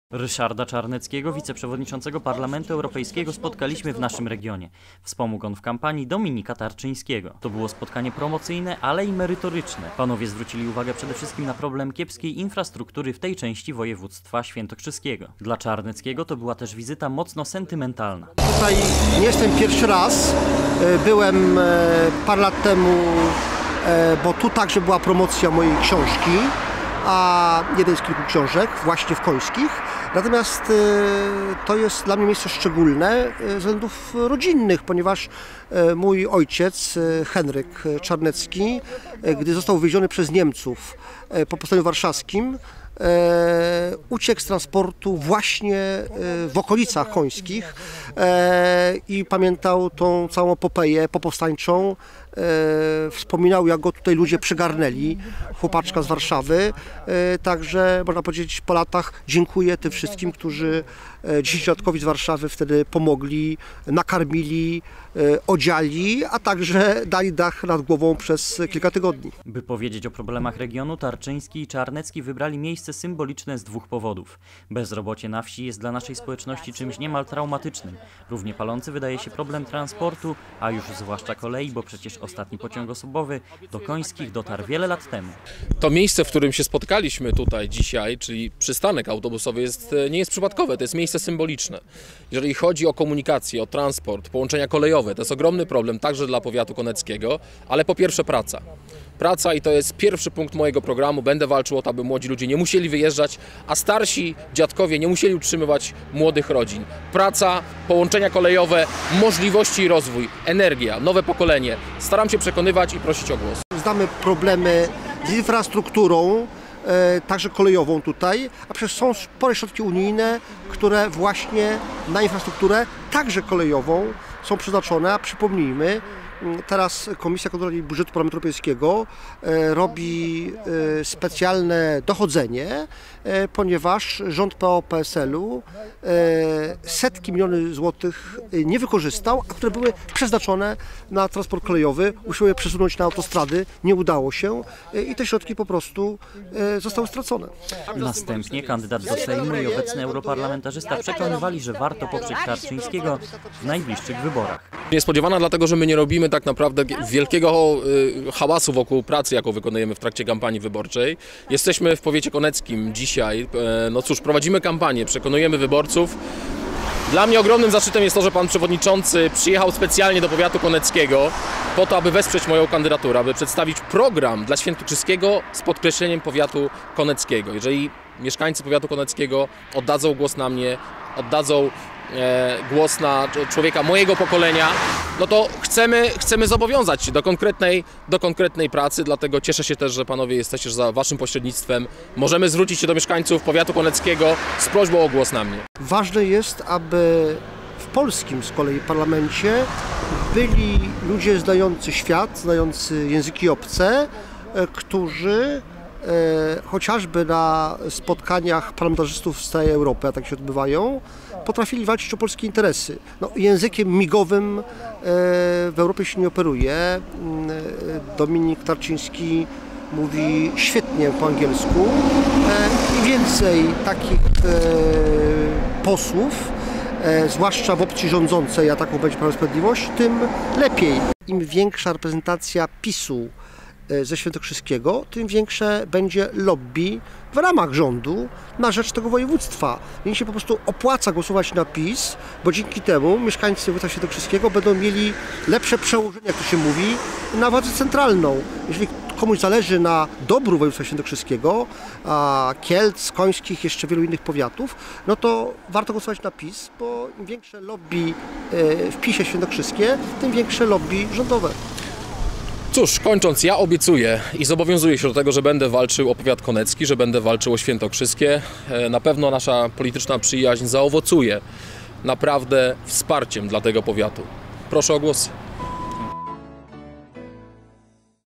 Ryszard Czarnecki w powiecie koneckim
Ryszard Czarnecki to jedna z głównych twarzy Prawa i Sprawiedliwości. Obecnie to wiceprzewodniczący Parlamentu Europejskiego. Wczoraj gościł na Konecczyźnie.